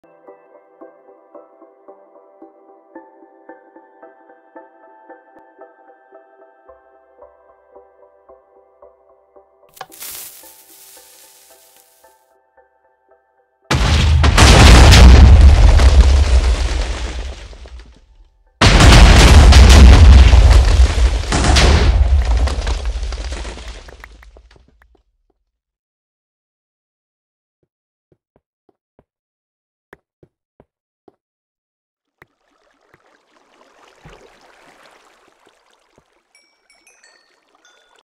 Things go boom :B
My friend Zane and I blowing up shit, seems I'm inclosed in the TNT :O